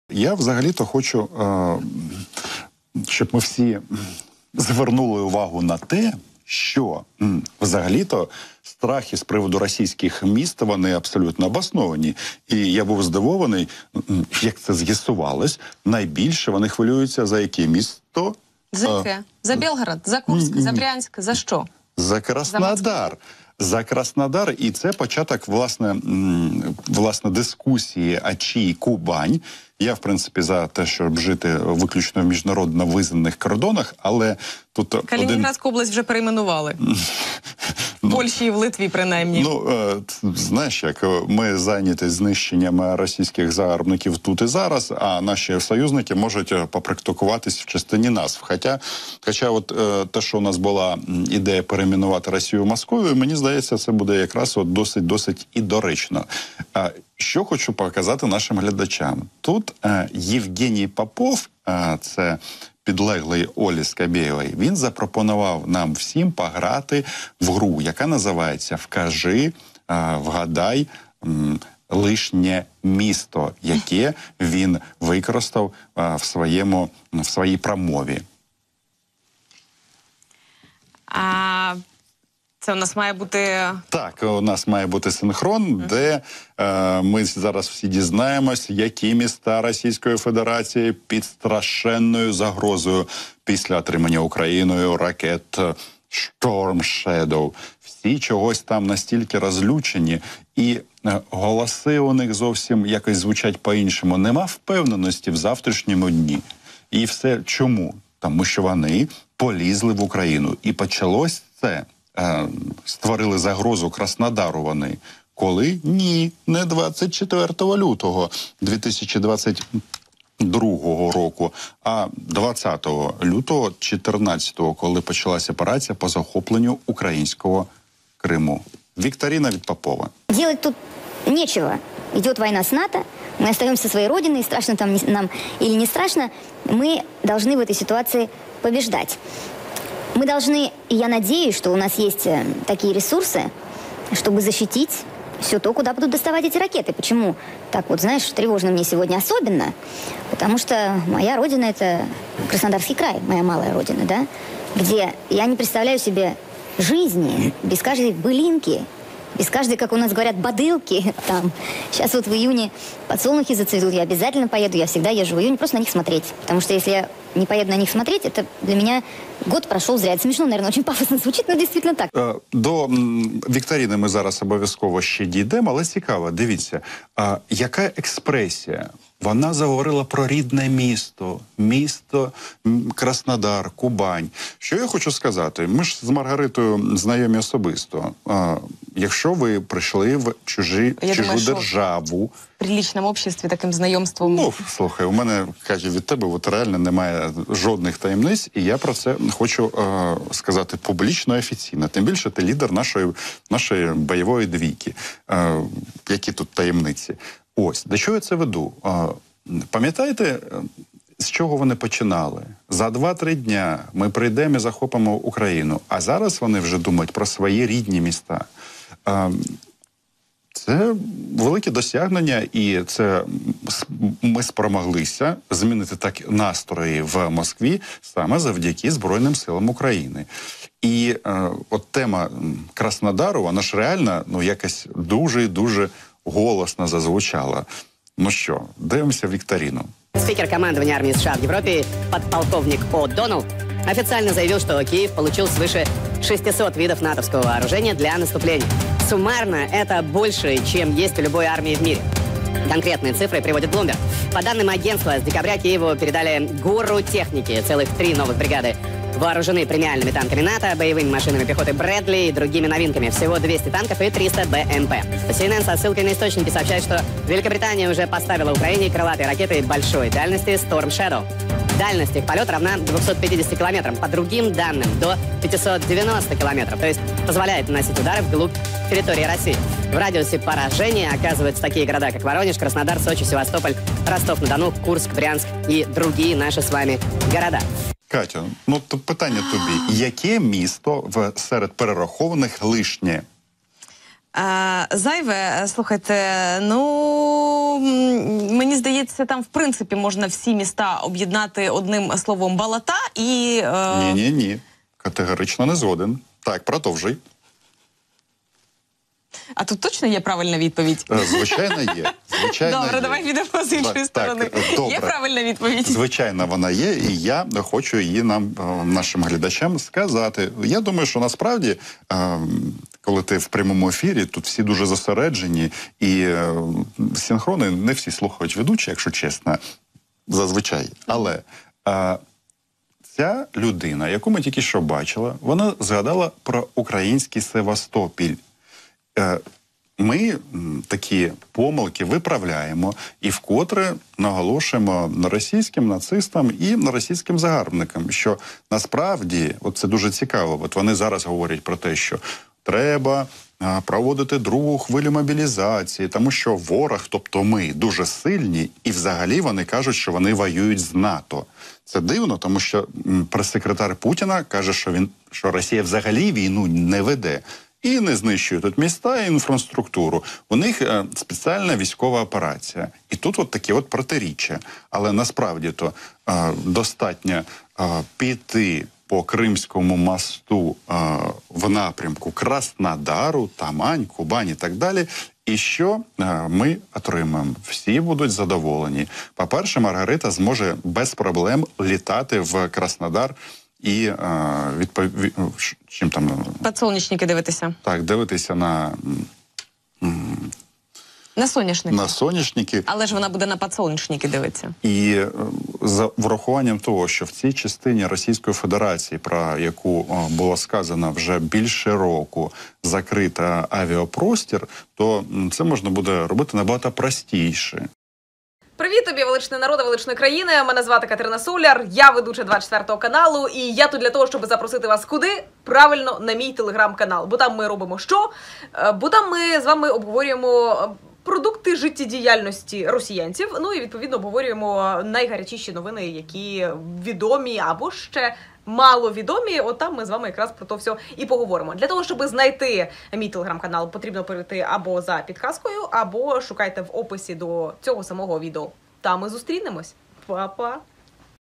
На Росії повітряні тривоги / Симоньян тікатиме / СОЛЯР та ЦИМБАЛЮК у захваті від паніки росіян
Велика Британія надіслала до України далекобійні ракети Storm Shadow. У різних модифікаціях їхня дальність сягає 250 та понад 300 кілометрів відповідно. Але у будь-якому випадку "ПВО" на Кримському мосту не завадить. В той час, російська пропаганда дуже панікує та переживає, що ракети зможуть долетіти до "глубінки" Росії.